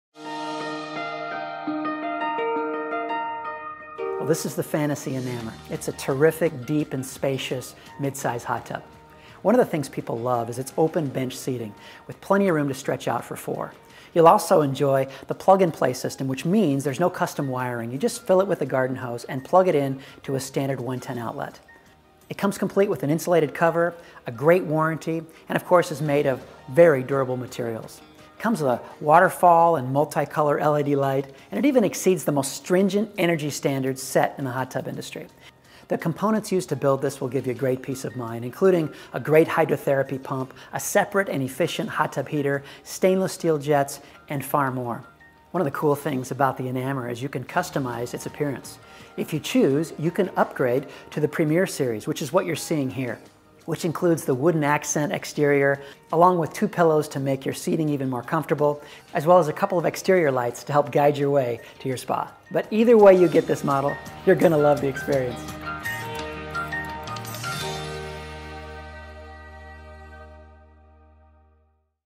Fantasy Spas - Enamor® Premier
The Enamor Premier offers the perfect escape from daily stresses and your todo list. With soothing hydrotherapy jets, comfortable pillows, and a spacious footwell, you can create a personal oasis in your own backyard! It’s a great way to have a date-night any day of the week without breaking the bank.

This 4-person, Plug-N-Play spa features an adjustable waterfall, exterior and interior multi-colour lights, and a built-in ice bucket that keeps your drinks cold for hours. The Enamor Premier makes it easy to take a break from your busy schedule and spend some much-needed quality time with your best friend.